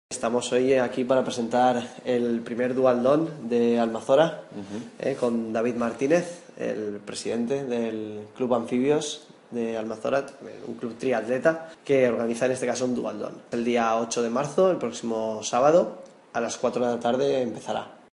Duatló Almassora
El regidor de Deportes en el Ayuntamiento de Almazora, Luis Molina, y el presidente del Club Triatlón Anfibios Almassora, David Martinez, han presentado esta mañana la primera edición del Duatlón Almassora. Organizado por el club triatleta local, en colaboración con el Ayuntamiento, este evento deportivo reunirá el próximo sábado, a partir de las 16.00 horas, a cerca de 300 atletas para participar en una competición que, tal y como ha destacado el edil, "permitirá que todos los amantes del deporte de la localidad disfrutemos de una prueba multidisciplinar nunca antes organizada en nuestro municipio".